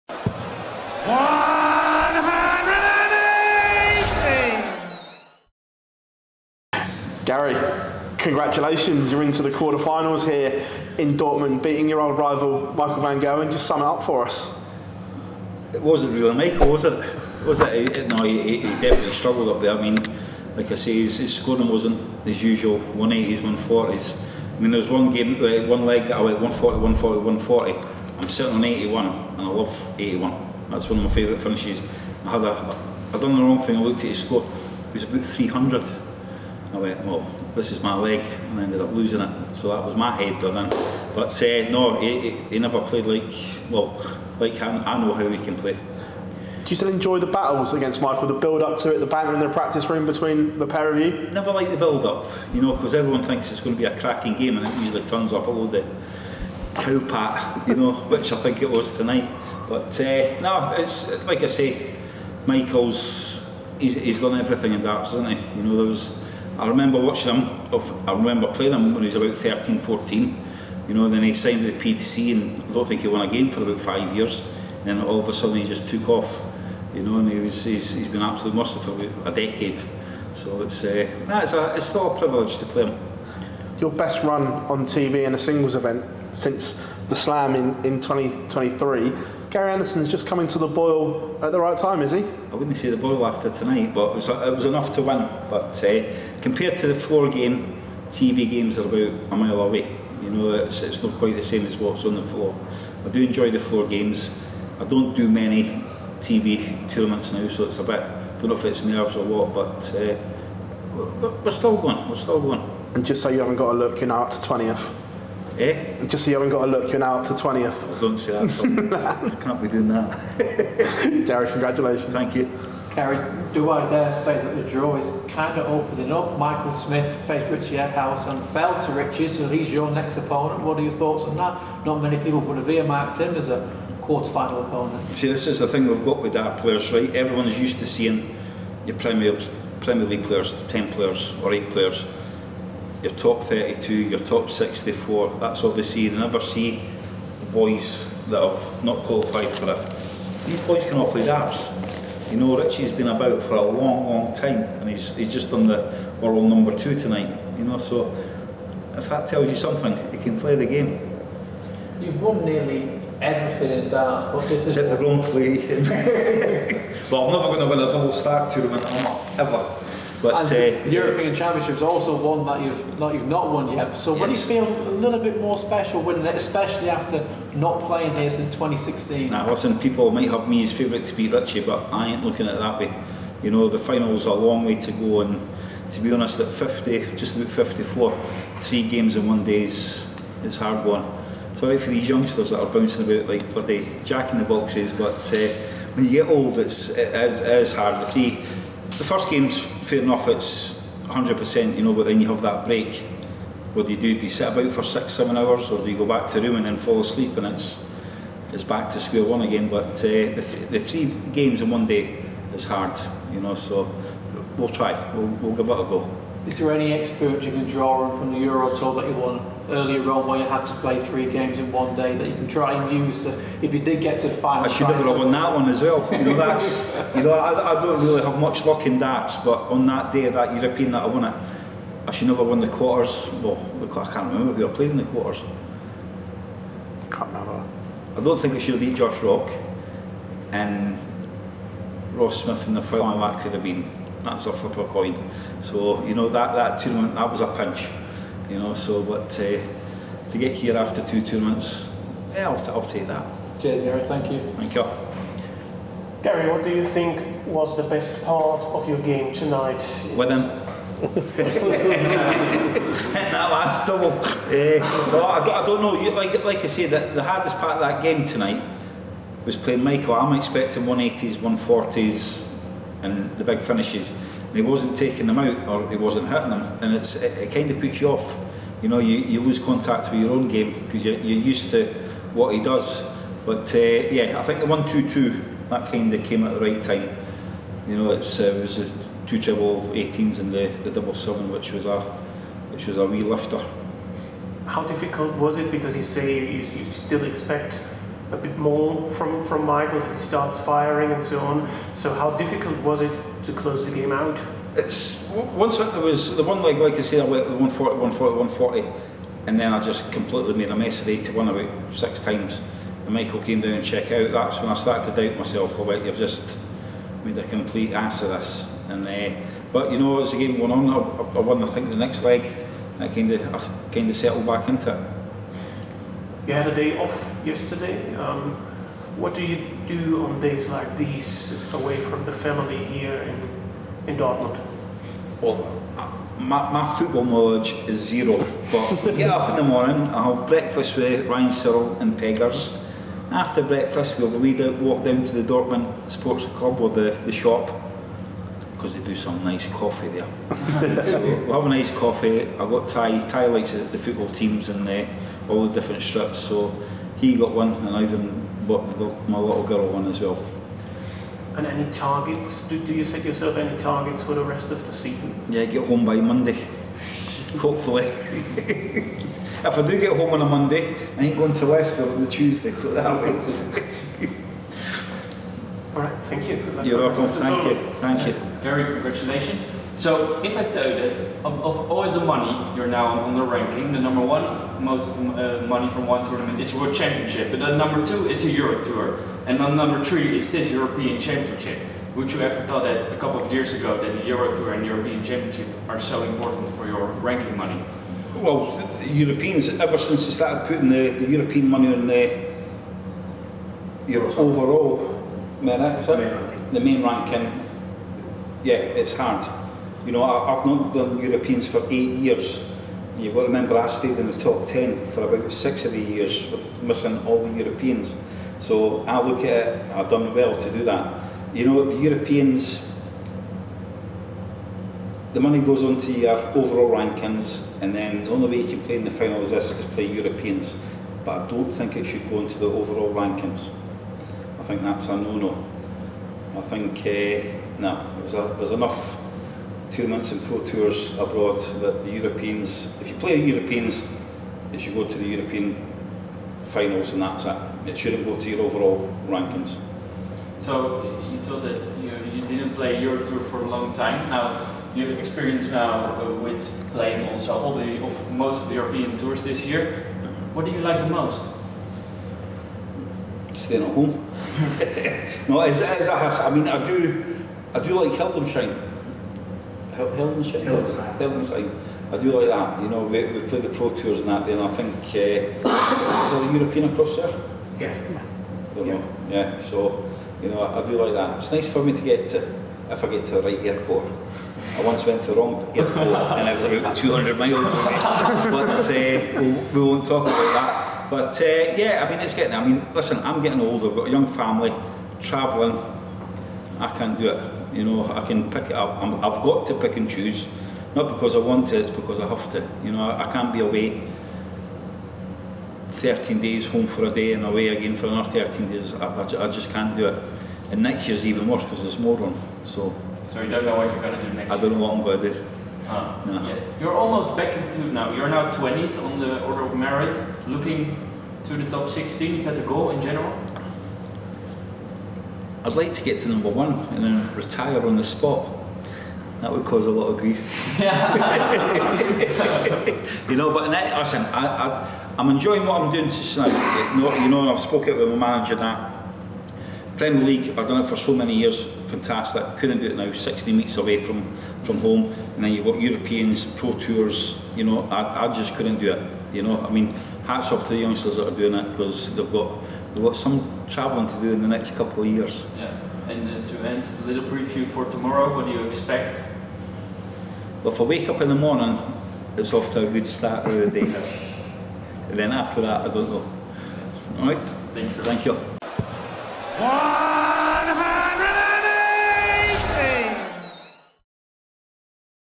"I'D LIKE TO GET TO NUMBER 1 AND RETIRE ON THE SPOT" GARY ANDERSON RULES OUT A PREMIER LEAGUE RETURN
Gary Anderson jokes about causing a lot of grief and again rules out a Premier League return "I'd like to get to number one and retire on the spot. That would cause a lot of grief.I've spoken with my manager, Premier League, I've done it for so many years, fantastic. Couldn't do it now"